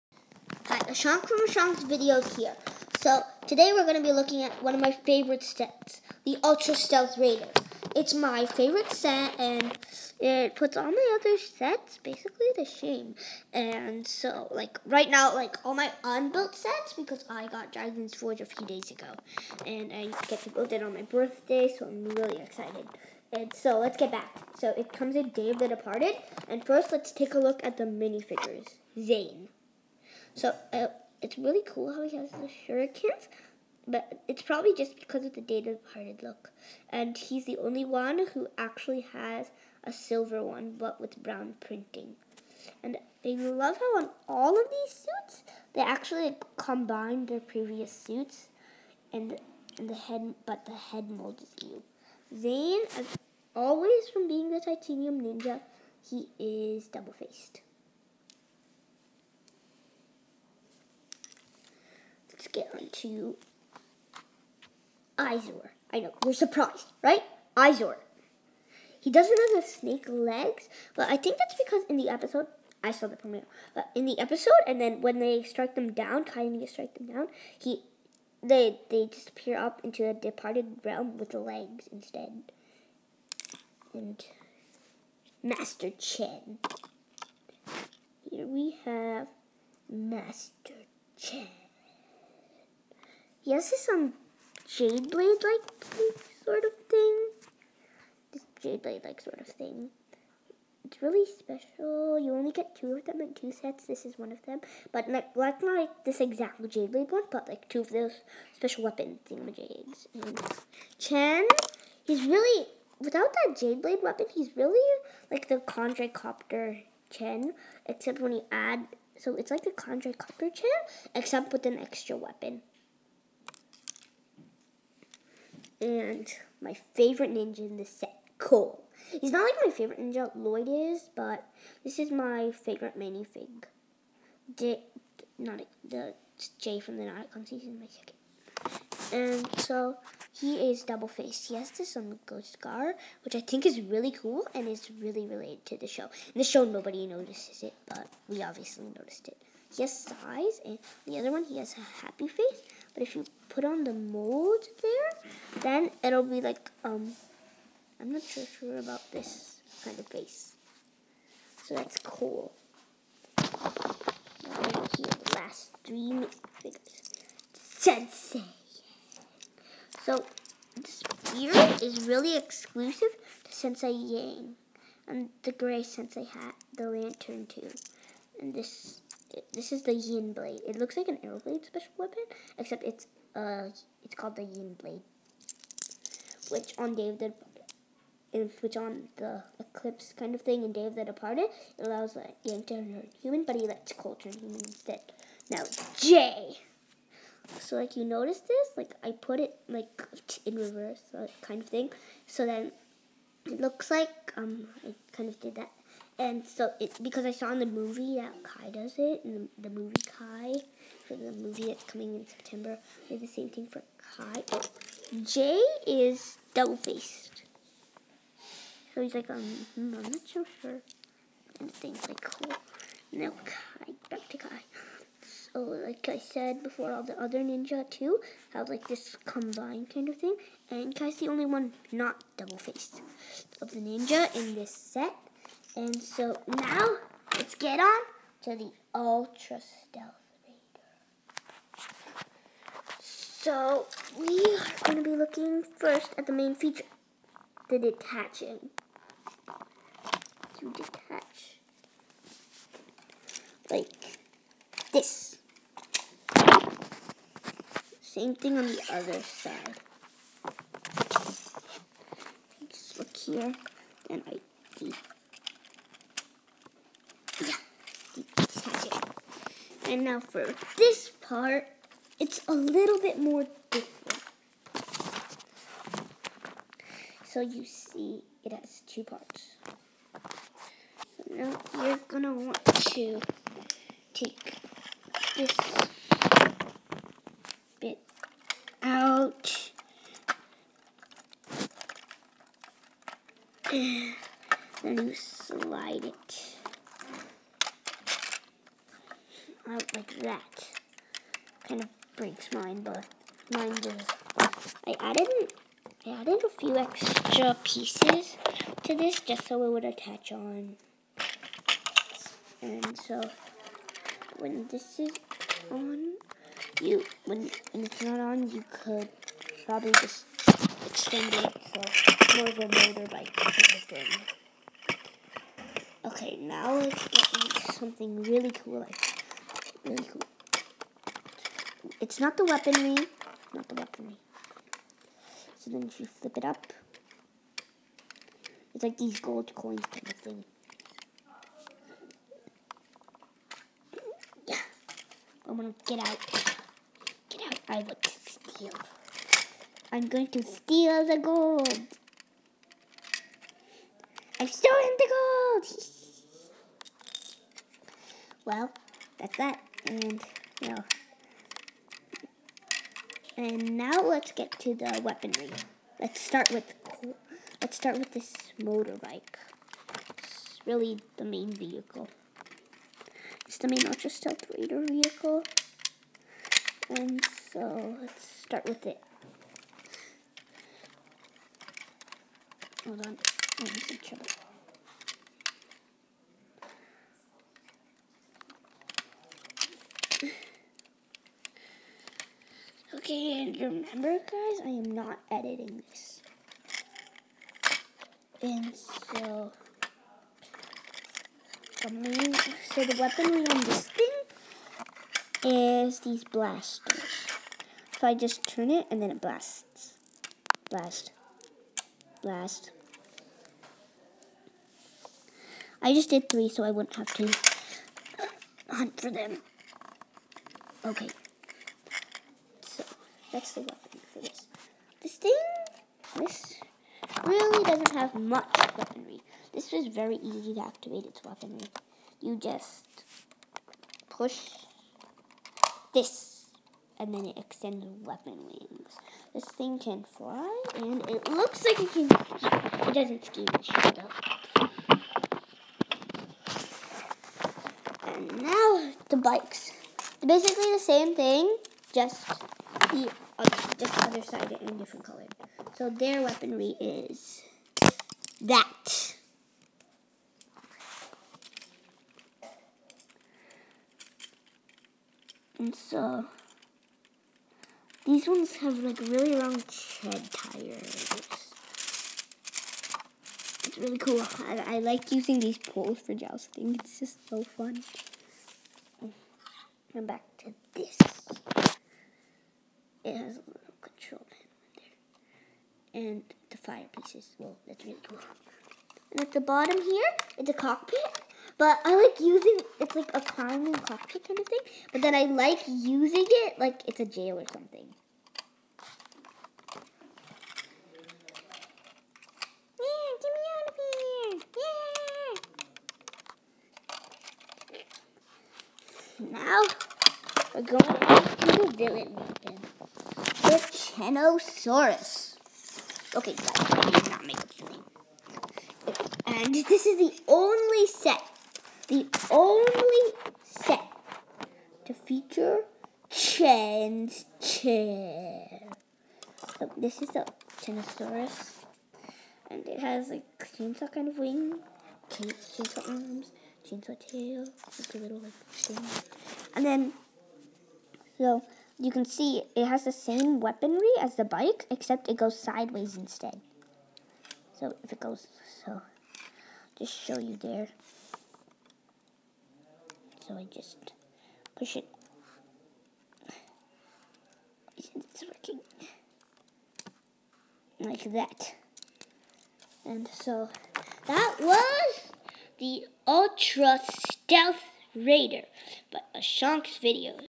The Ultra Stealth Raider!
Review on the ultra stealth raider from Ninjago.